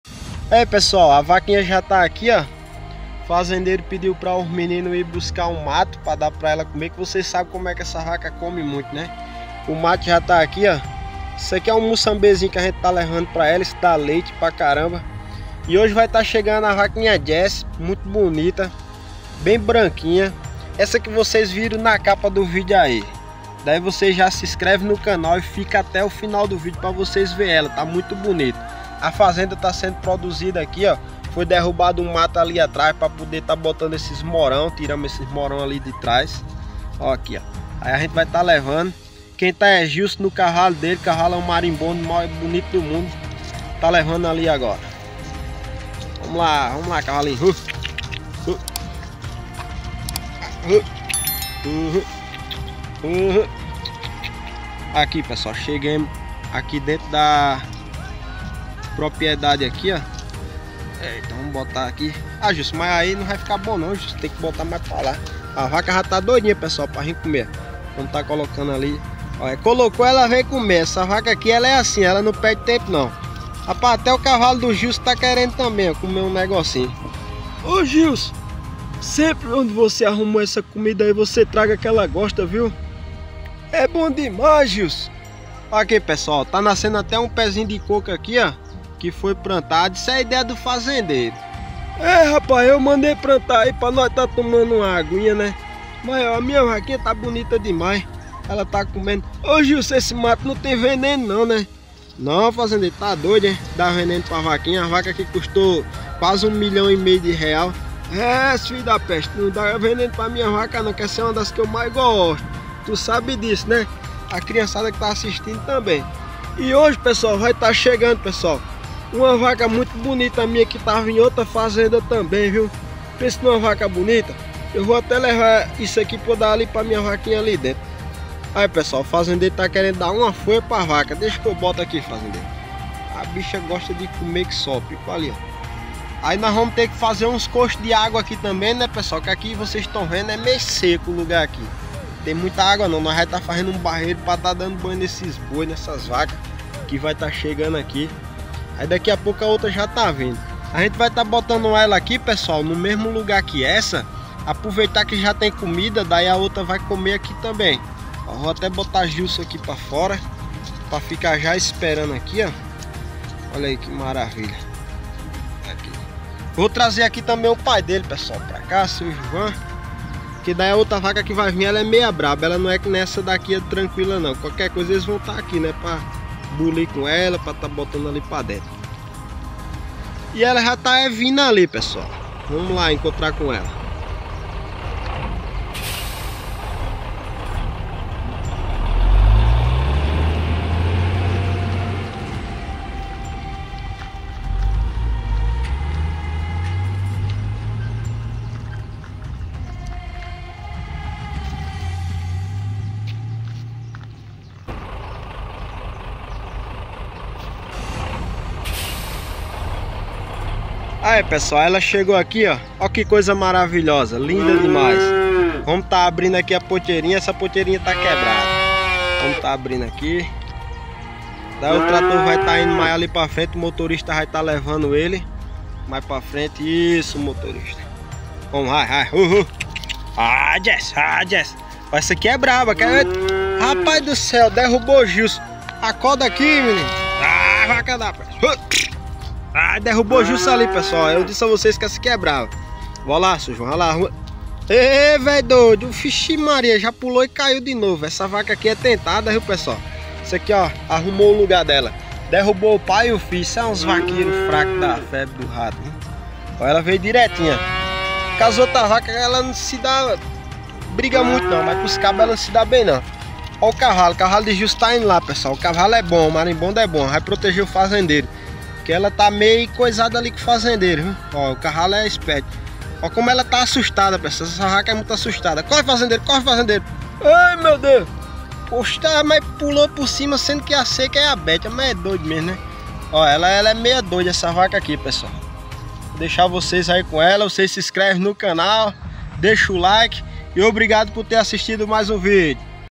chegou a vaca Jesse na fazendinha de brinquedo
chegou a vaca Jesse na fazendinha de brinquedo, está vaca ela é muito brava , hoje estava está chegando a nossa fazenda junto com a vaca africana, venha ver o vídeo e se surpreenda,